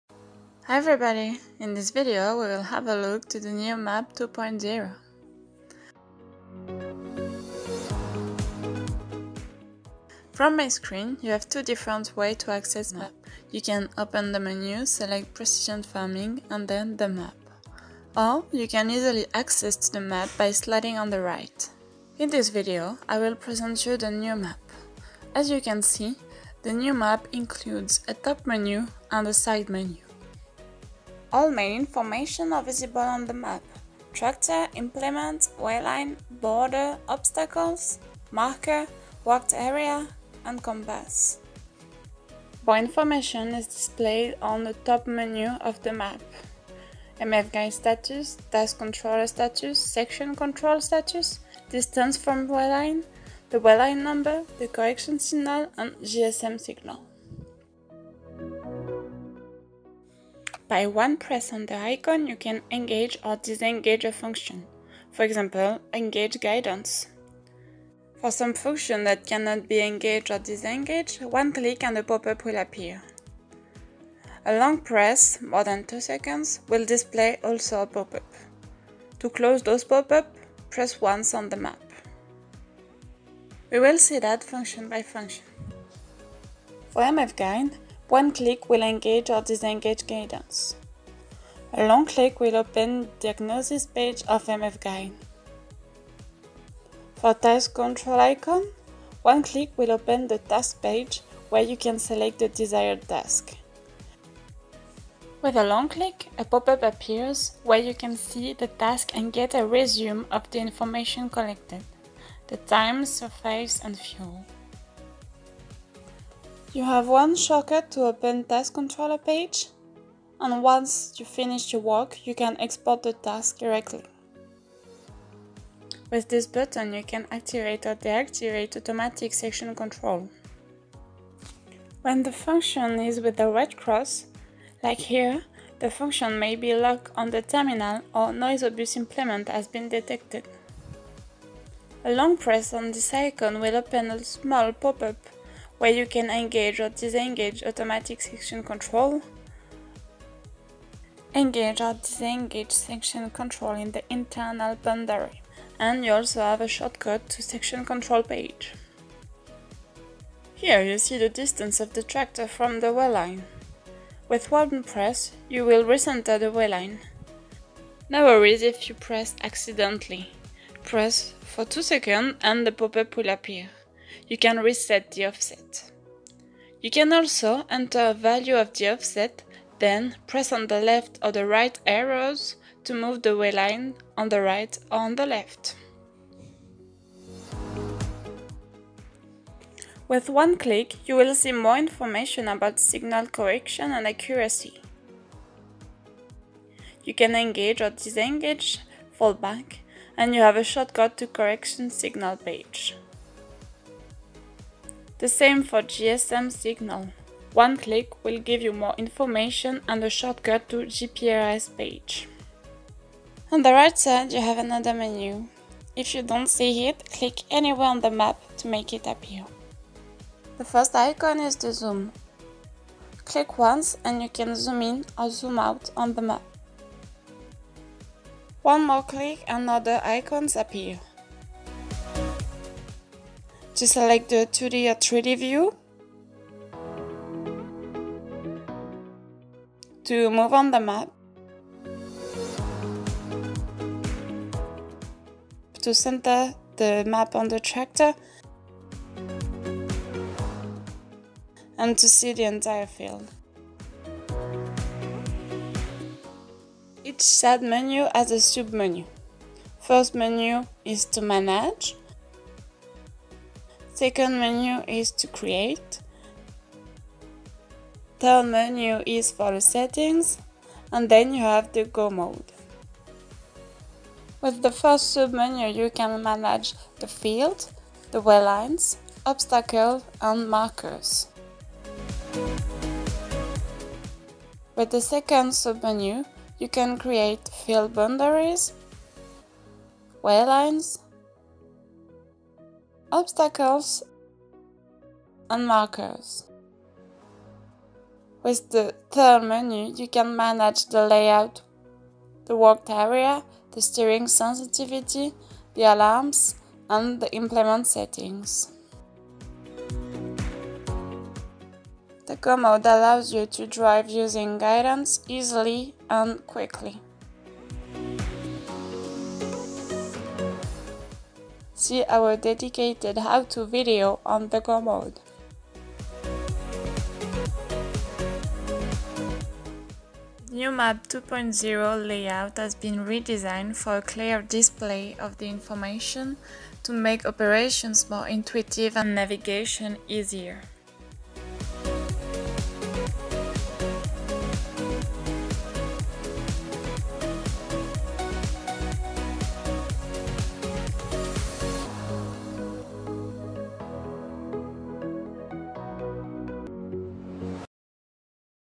MF TERMINALS | HOW TO | USE MAP 2.0
In this video, we will have a look at the new Map 2.0. The new map includes a top menu and a side menu. All main information is visible on the map: tractor, implement, wayline, borders, obstacles, marker and worked area. And the new map displays more information on the top menu. You have the MF Guide status, Task Controller status, Section Control status, distance from wayline, the wayline number, the correction signal and GSM signal.